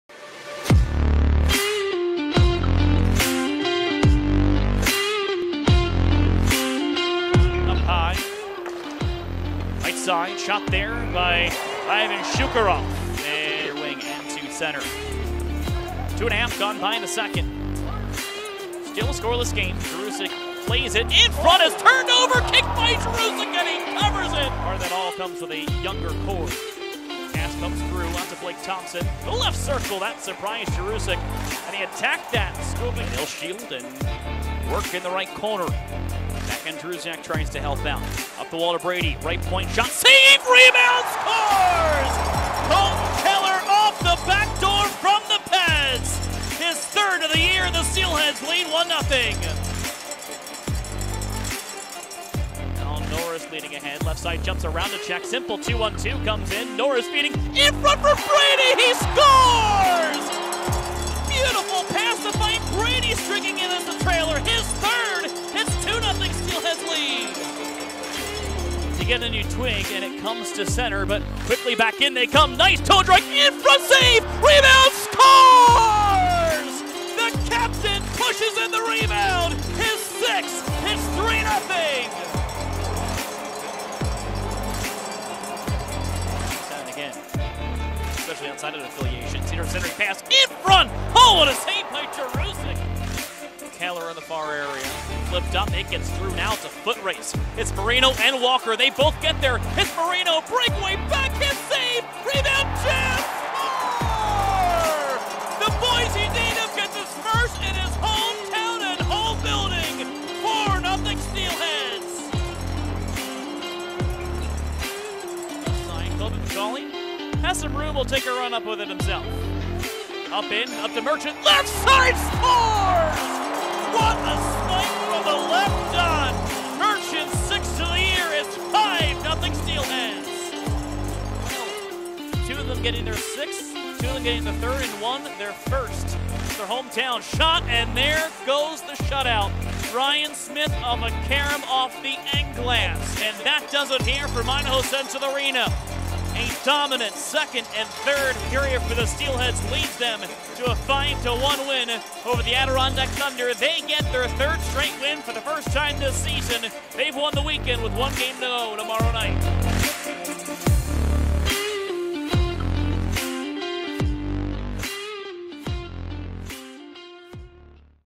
ICCU Postgame Highlights: Adirondack vs Idaho (11/19/2021)
No better way to start the weekend than with another big win on home ice!

Check out the Idaho Central Credit Union Postgame Highlights from last night's victory as we get set to wrap up our three game series with the Adirondack Thunder tonight!

See the Steelheads live!